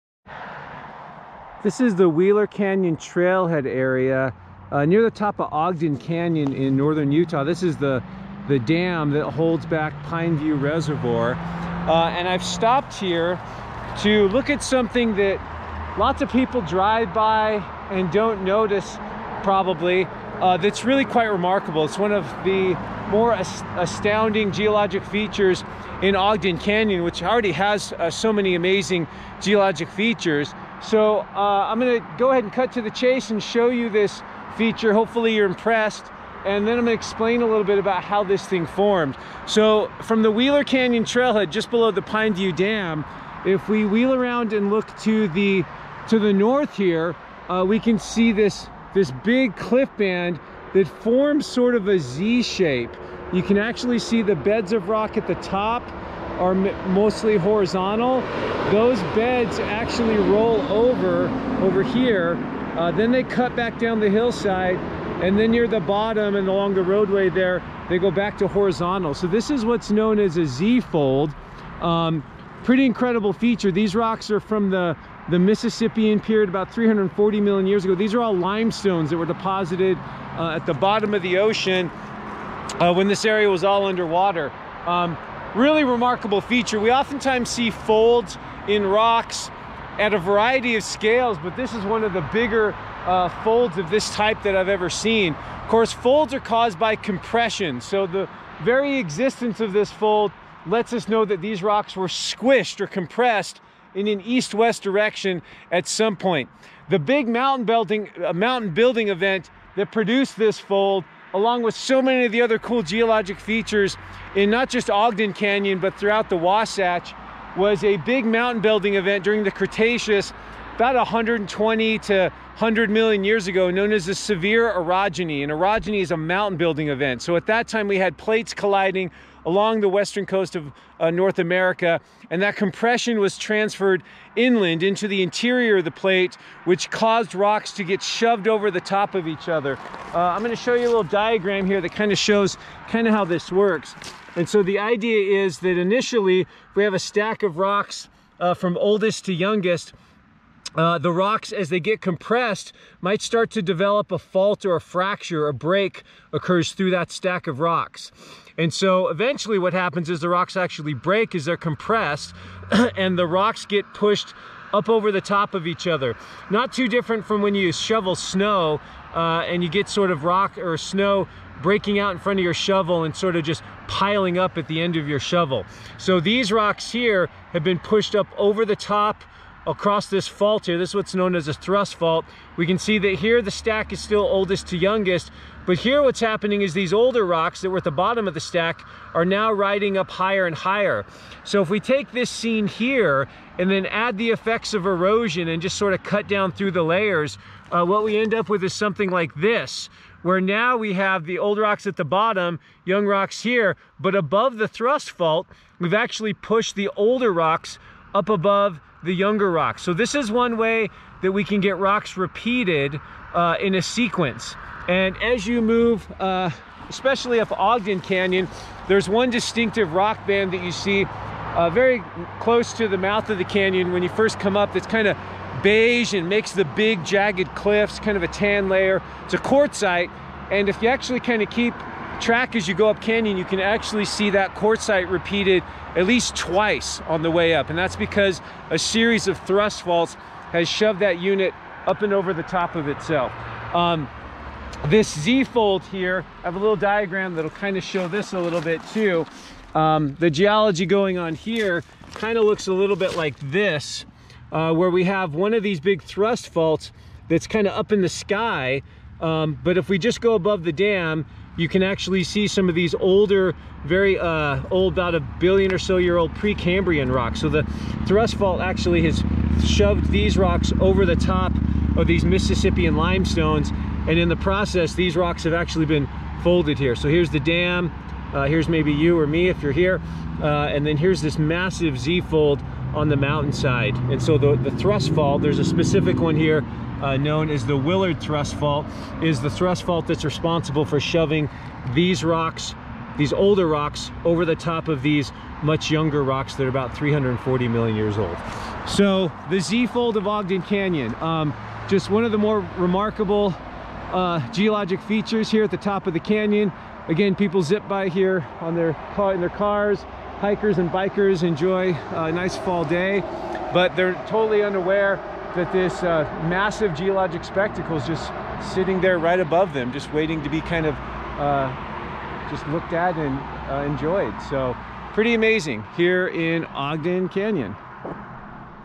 The fantastic and massive Z-fold at the Wheeler Canyon trailhead in Ogden Canyon, Utah
While motorists zip by on Utah Highway 39, geology professor Shawn Willsey shares the story of the awesome Z-fold on full display just north of the Wheeler Canyon trailhead. This huge fold was caused by east-west compression about 100 million years ago during the Sevier Orogeny, a mountain building event that affected much of Utah and adjacent states.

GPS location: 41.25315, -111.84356

or click on the three dots below the viewer then "Thanks"

or a good ol' fashioned check to:
Shawn Willsey
148 Blue Lakes Blvd N